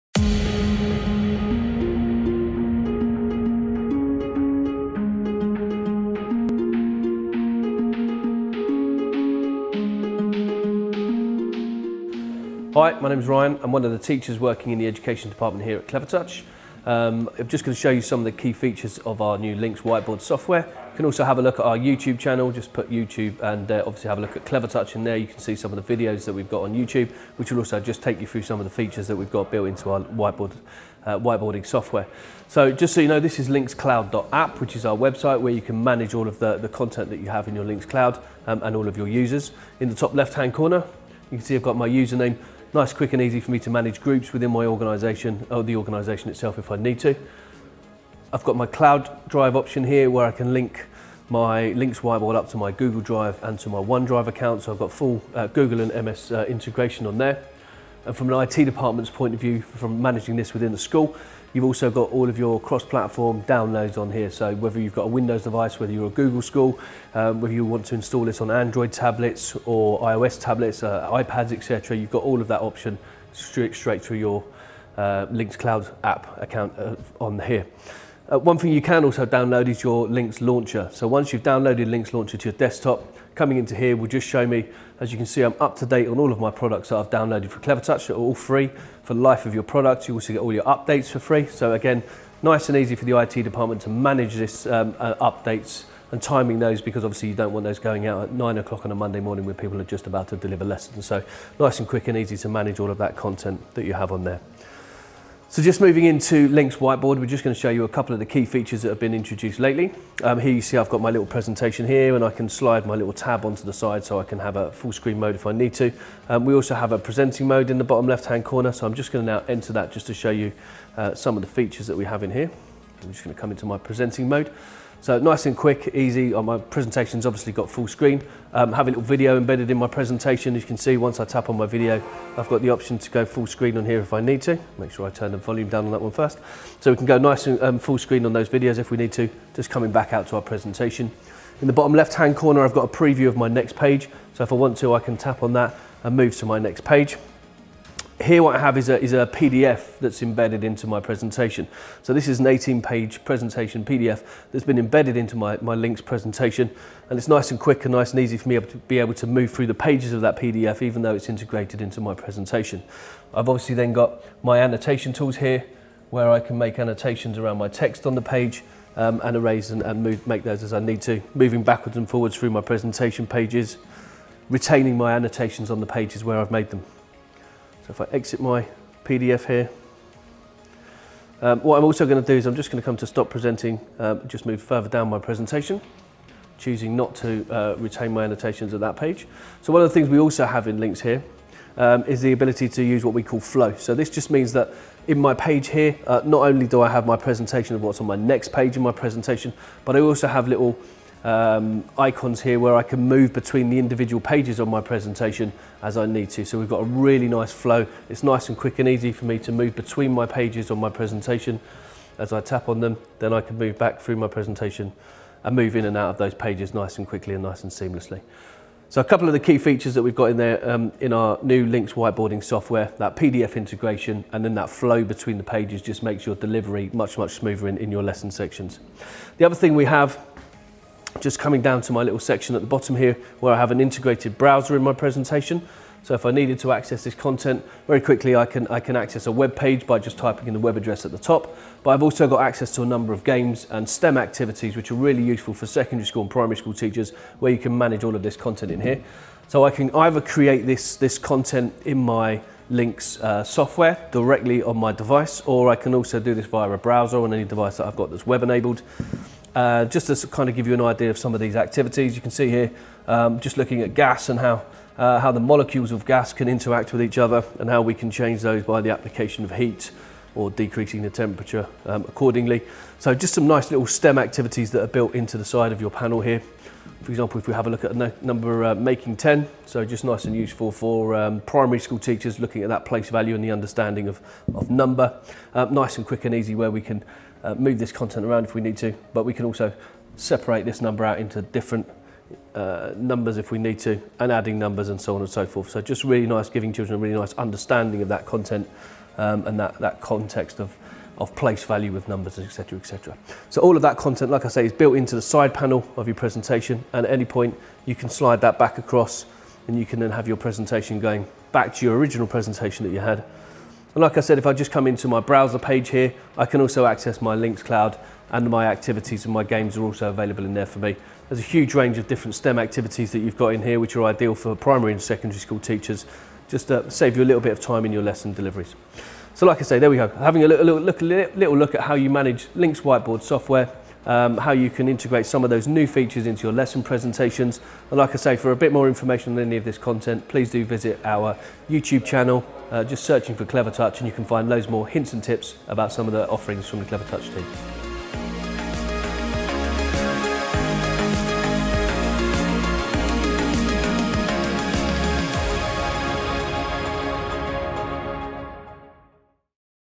Clevertouch | LYNX Whiteboard | What’s New
Introducing to you the new features of LYNX Whiteboard, including embedded video and playing them in full screen within the presenter mode or moving seamlessly between pages with the flow.